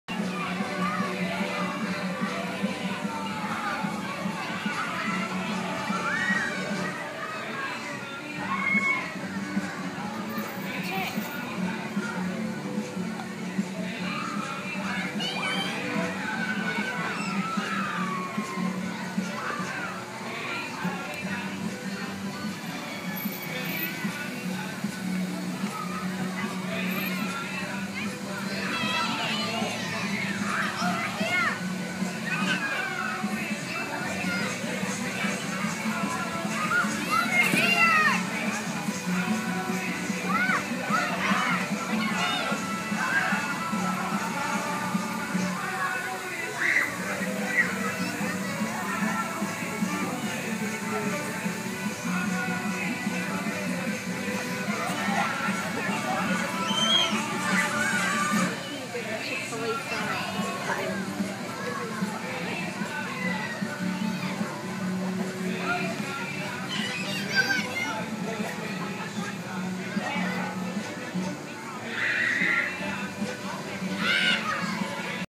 Coolest Chick-Fil-A in Tampa FL
Christmas display at Chick-Fil-A.